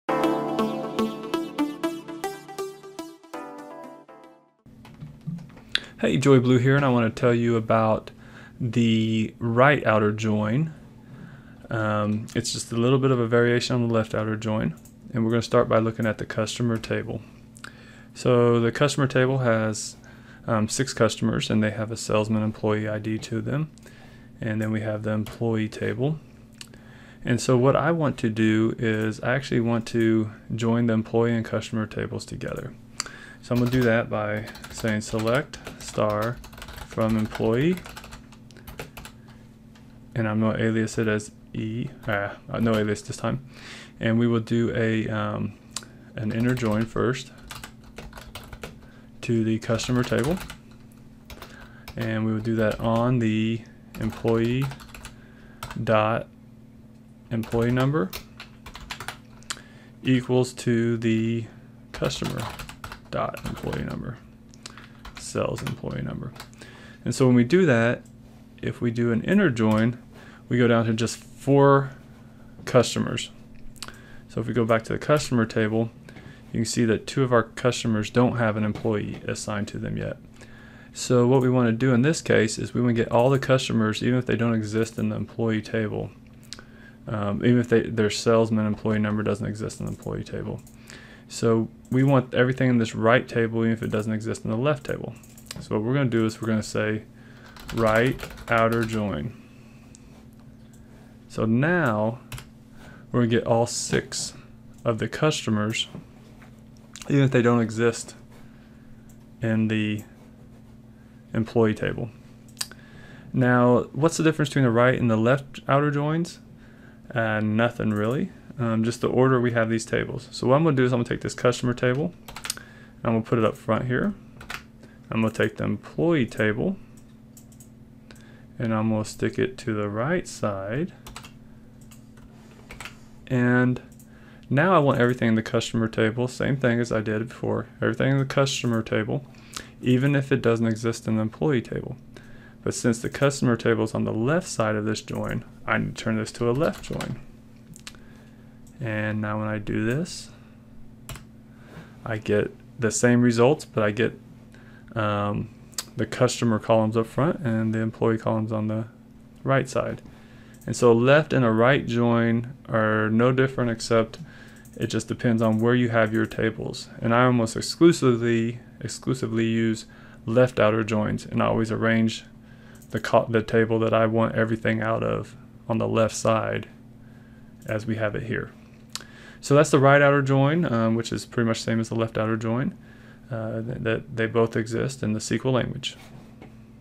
SQL Right Outer Join - SQL Training Online - Quick Tips Ep26
Learn about the Right Outer Join in the SQL Language.